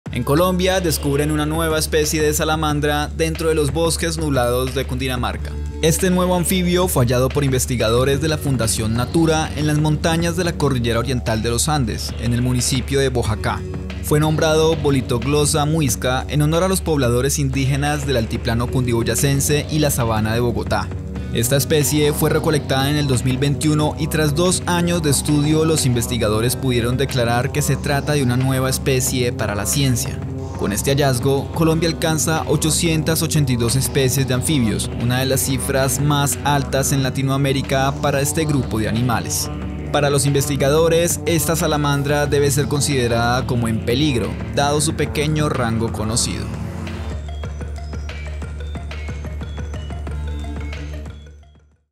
Colombia: descubren nueva especie de salamandra en los bosques nublados de Cundinamarca
🇨🇴🦎 Comenzamos el día con buenas noticias. Investigadores descubrieron una nueva especie de salamandra en los bosques nublados de Cundinamarca. Con este hallazgo, Colombia alcanza 882 especies de anfibios, una de las cifras más altas en Latinoamérica para este grupo de animales. 🥳 ¿Qué características tiene esta especie?